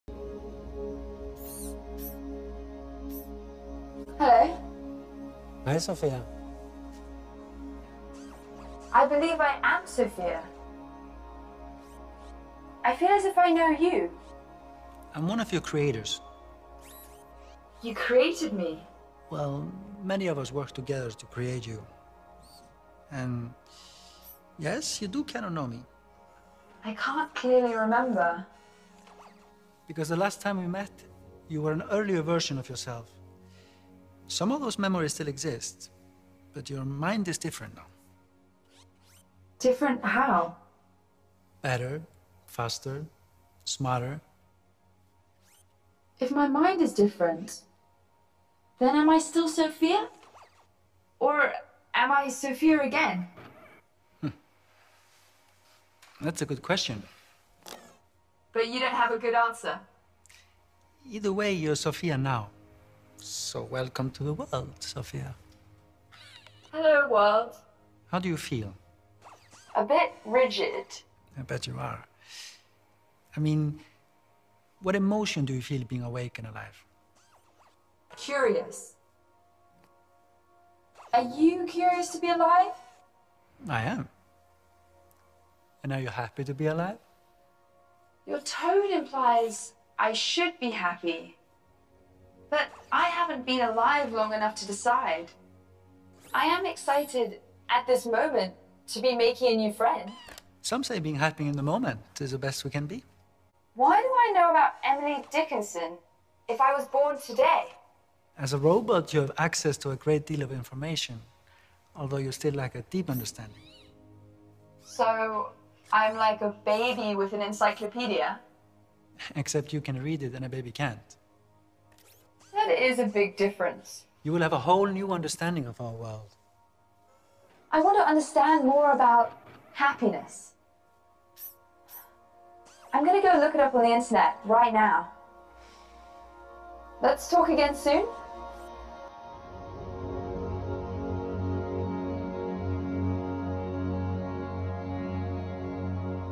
ذكائها يتطور بشكل مخيف !! شاهد كيف تتحدث الروبوت صوفيا - مترجم
ذكائها يتطور بشكل مخيف!! شاهد كيف تتحدث الروبوت صوفيا